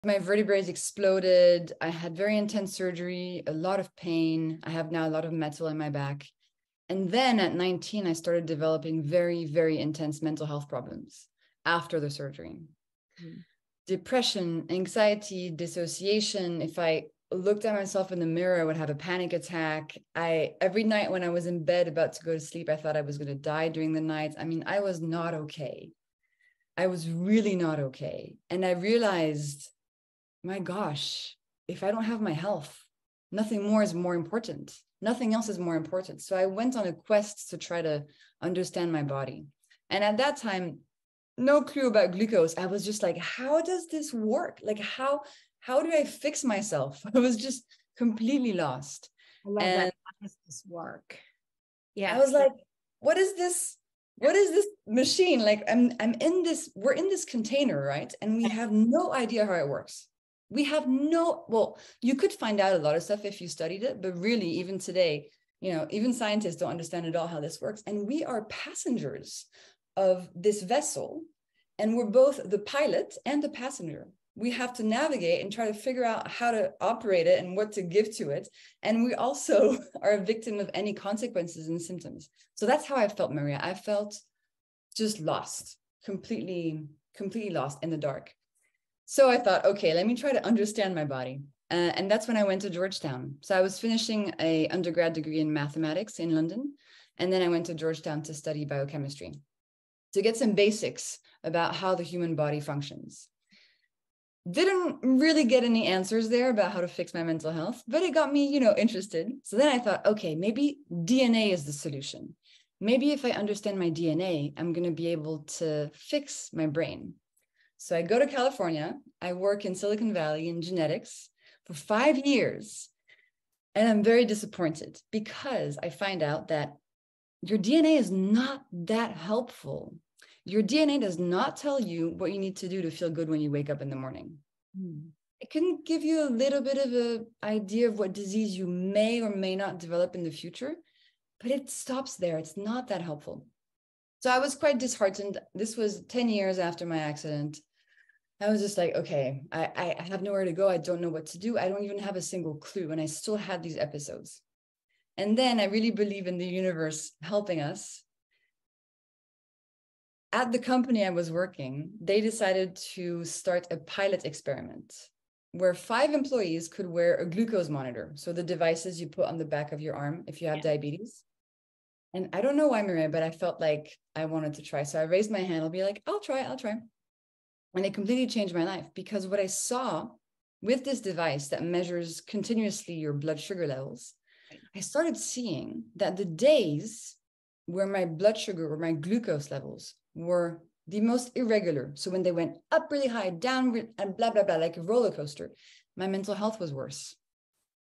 Jessie Inchauspé "The Glucose Goddess" on Hacking Your Blood Sugar for Your Health
In the conversation, Inchauspé talks with Maria about the simple hacks backed by cutting-edge science that we can use to optimize our blood sugar and regain power over our wellness. She dives into how she became the Glucose Goddess, how blood glucose is connected to our brain and overall health, and the best foods to eat (and when) to help us keep our glucose level in check.

Conversations Above the Noise with Maria is a regular series that invites Sunday Paper PLUS members to attend live conversations with true Architects of Change.

For more inspiring content from the greatest hearts and minds, you can visit The Sunday Paper